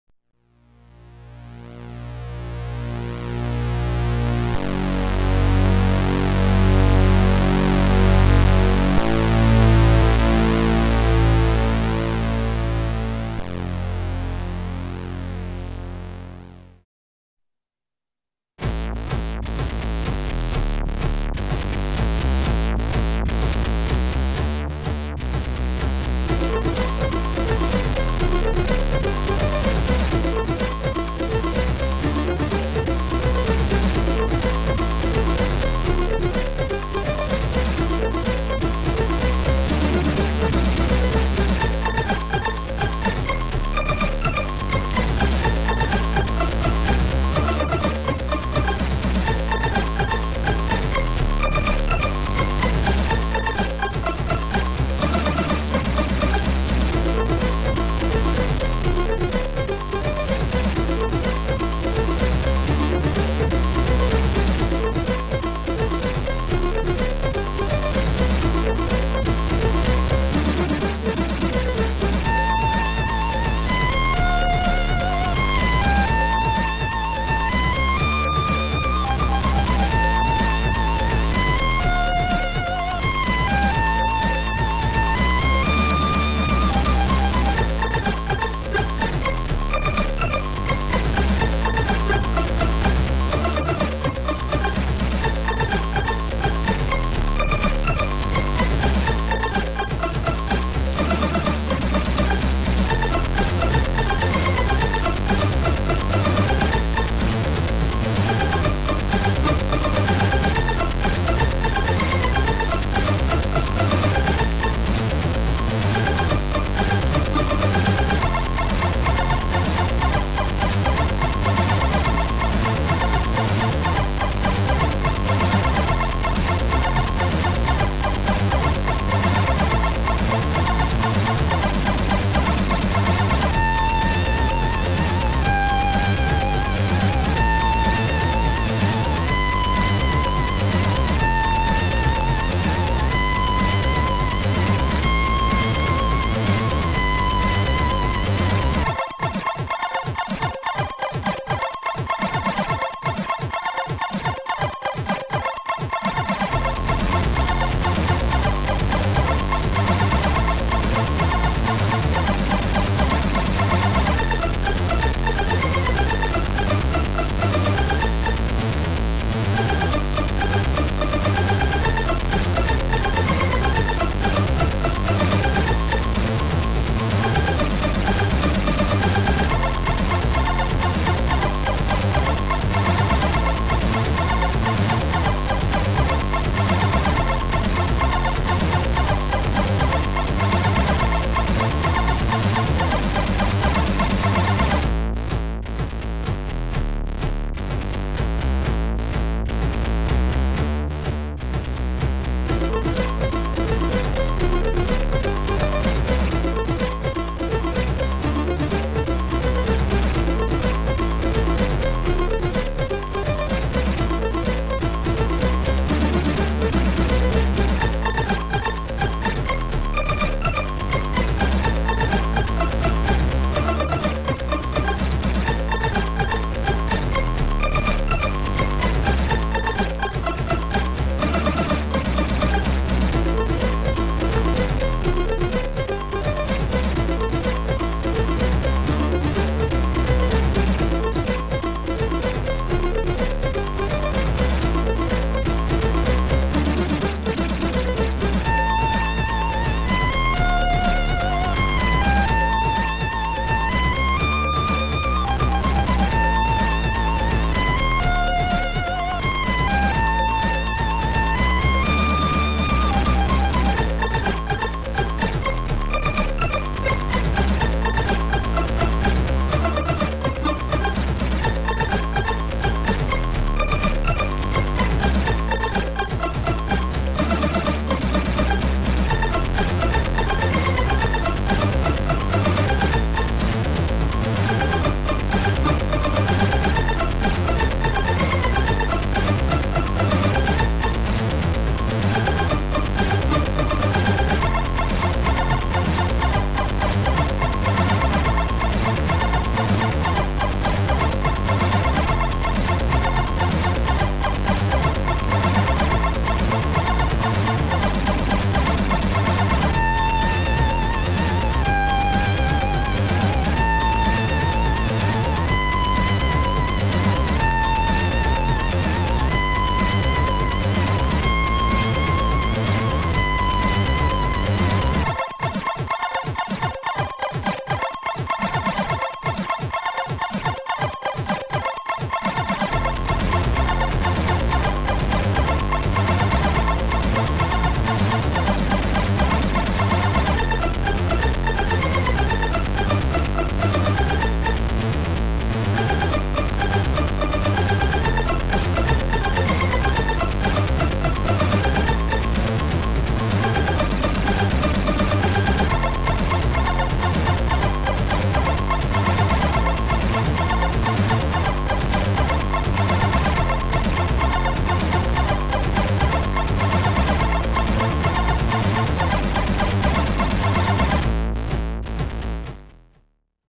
Fantasy Intro by Fantasy - Commodore 64 Cracktro
Fantasy Intro, released 25 May 1989

Credits:
Code: Mr Neutron ( of Havok )
Graphics: The Punisher ( of Pyramania )

Disclamer: DLG doesn't support Piracy, this demo is for entertaiment only